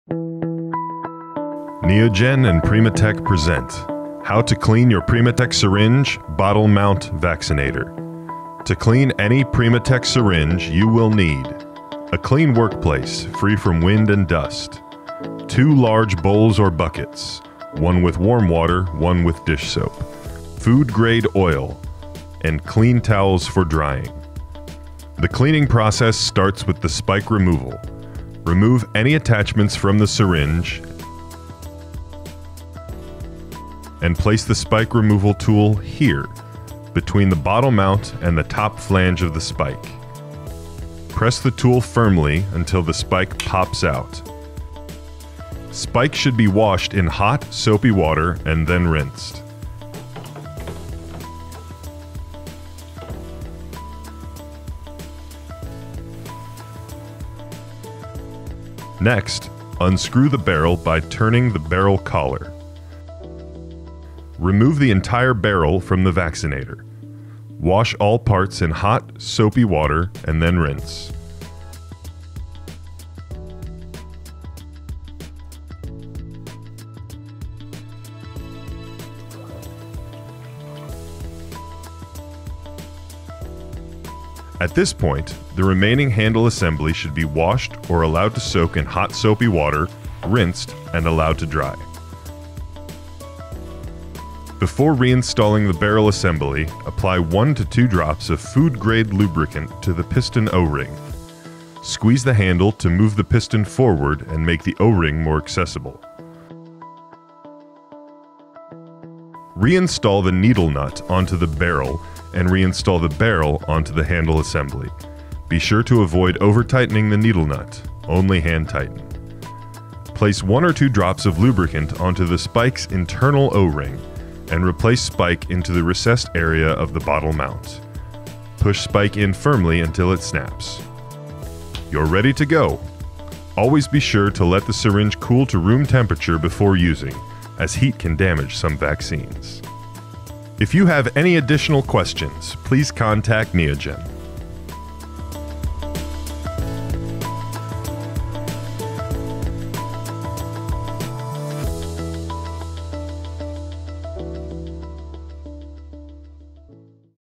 How to Clean Your Prima® Syringe | Bottle Mount Vaccinator
This video details how to clean your Prima® bottle mount vaccinator.